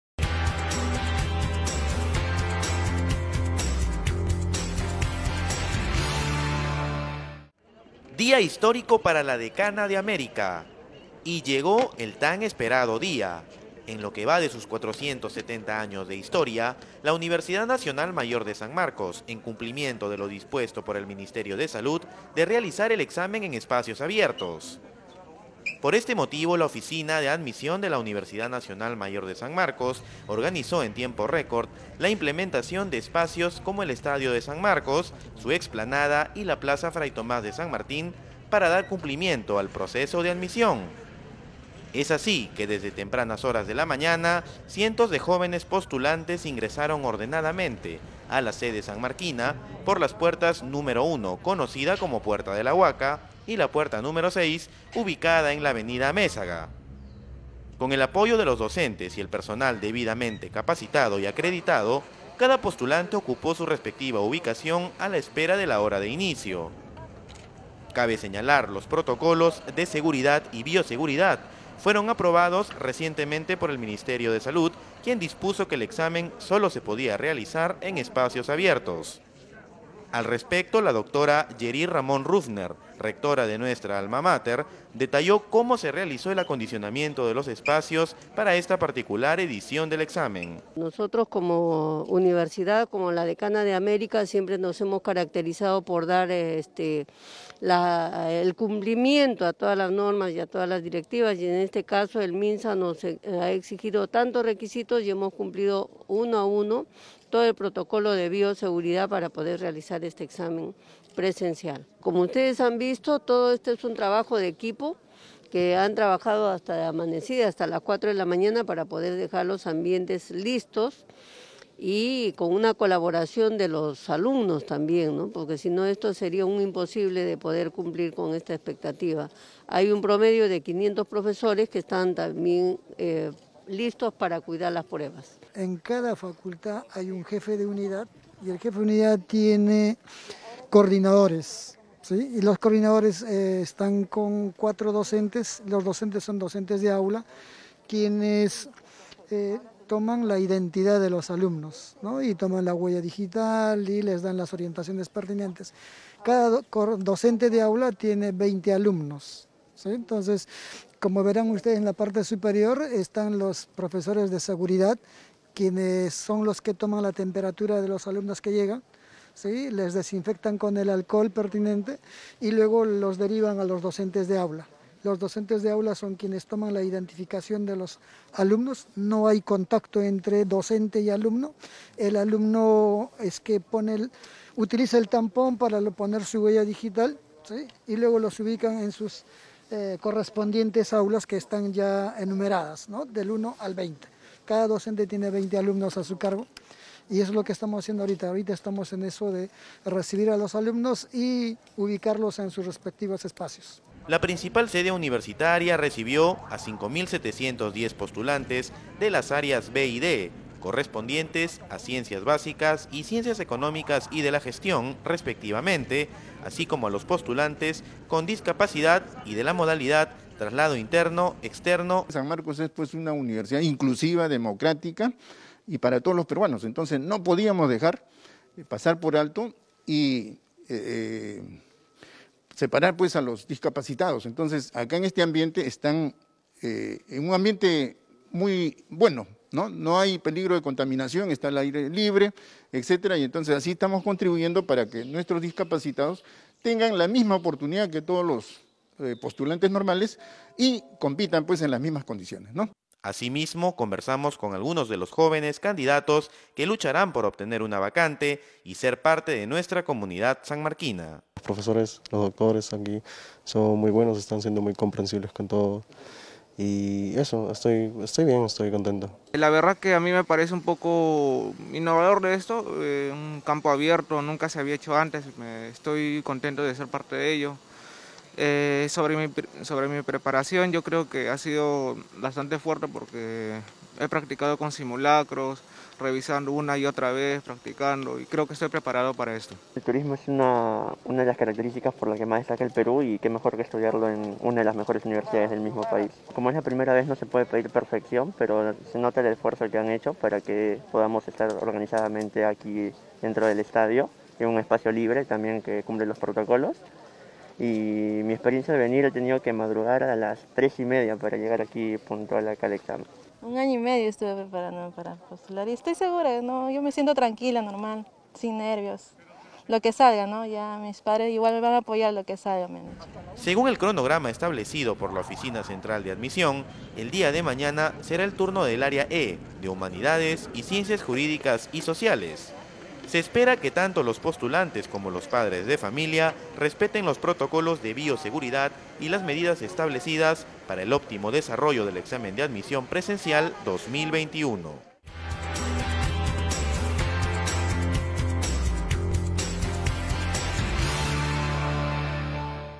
Resumen: Primer Día Examen de Admisión Presencial 2021 UNMSM 📚🖥️
Examen de Admisión Presencial se realizó cumpliendo todos los protocolos de bioseguridad recomendados por el Ministerio de Salud (MINSA)
Una vez más la Universidad, Decana de América, cumple con sus postulantes y con sus familias. Mañana Continuarán los postulantes de las áreas de Humanidades y Ciencias Jurídicas y Sociales
▬▬▬▬▬▬▬▬▬▬▬ஜ۩۞۩ஜ▬▬▬▬▬▬▬▬▬▬▬▬▬▬
Únete a Nuestro Grupo Pre✨
▬▬▬▬▬▬▬▬▬▬▬ஜ۩۞۩ஜ▬▬▬▬▬▬▬▬▬▬▬▬▬▬
Descarga todos los Materiales Pre✨